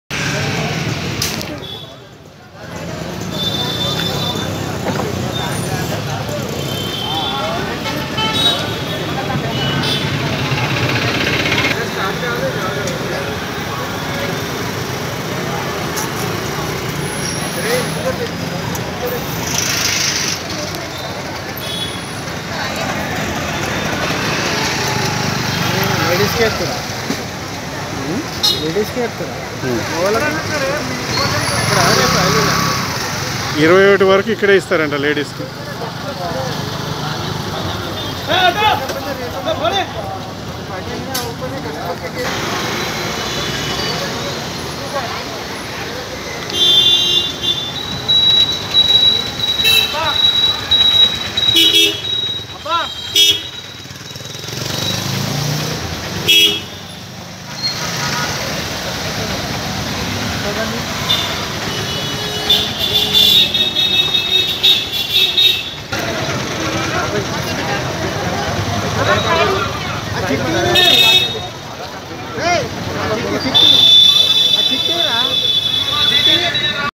free covaxin camp till jun21 for woman 18 plus
free ghmc covaxin camp west maredpally hurry every body utlize no rush vaccination with in 15min timing 8 am to 2pm
ATV NEWS Channel for unbiased news and aggressive journalism
For public grievances or complaints call Kphb Hyderabad ph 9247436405
Anti corruption helpline ph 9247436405